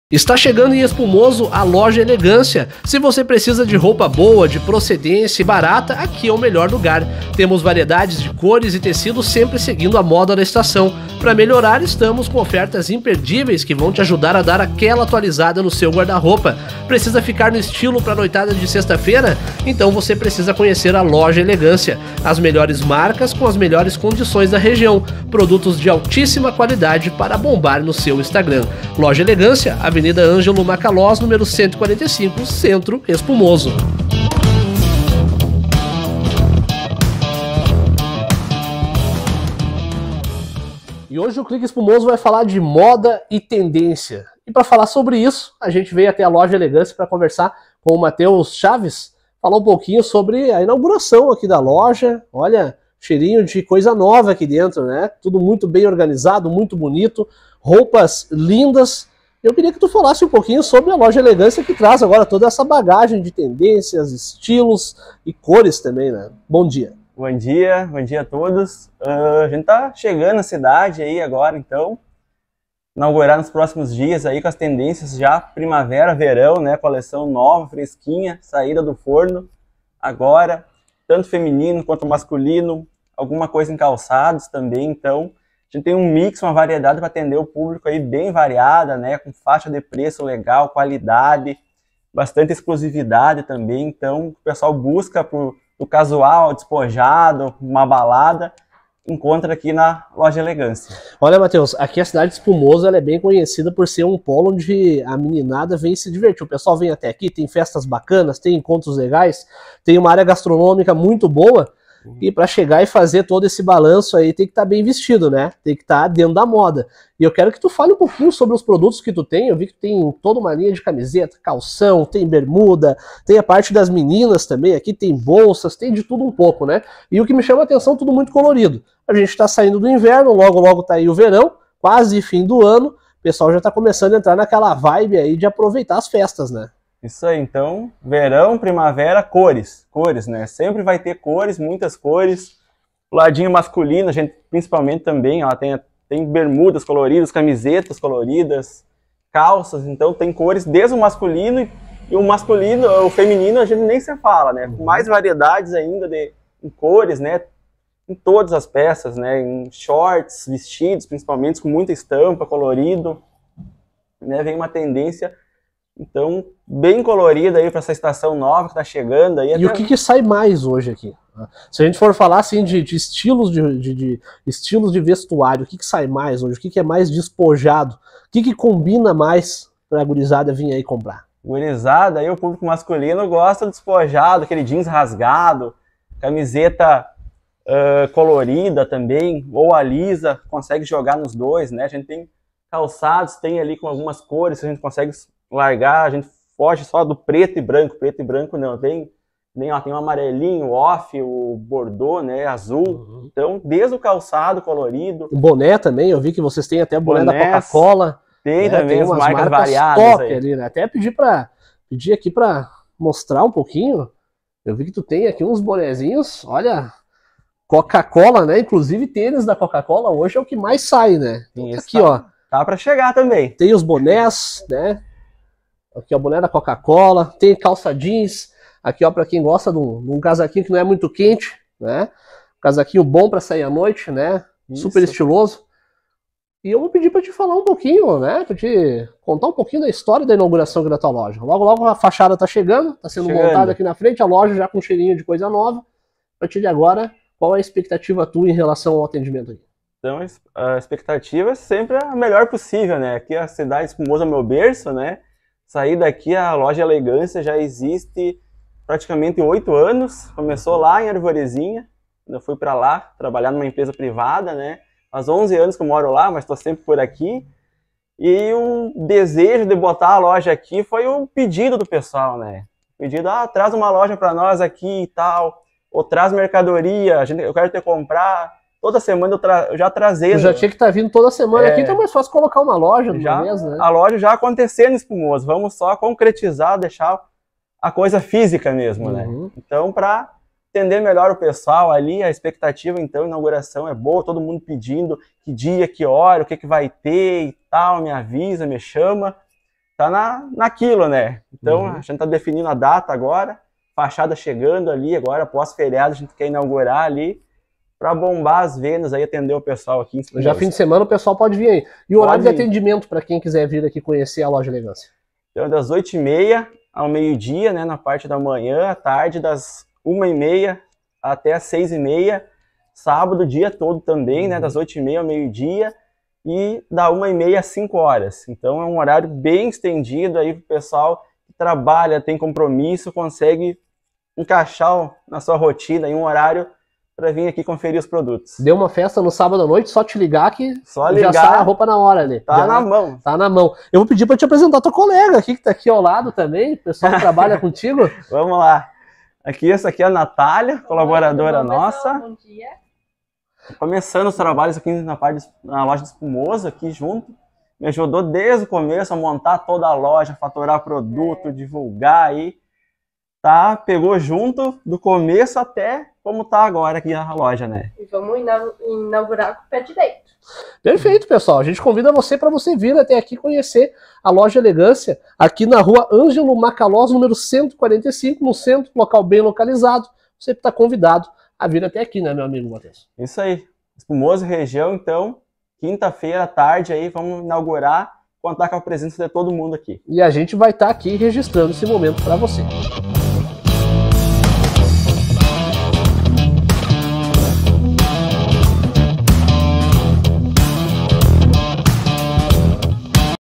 Está chegando em Espumoso, a Loja Elegância
Se você precisa de roupa boa, de procedência e barata, aqui é o melhor lugar! Temos variedade de cores e tecidos, sempre seguindo a moda da estação. Para melhorar, estamos com ofertas imperdíveis que vão te ajudar a dar aquela atualizada no guarda-roupa. Precisa ficar no estilo para a noitada de sexta? Então você precisa conhecer a Loja Elegância! As melhores marcas com as melhores condições da região. Produtos de altíssima qualidade para bombar no seu Instagram.
Com informações: ClicEspumoso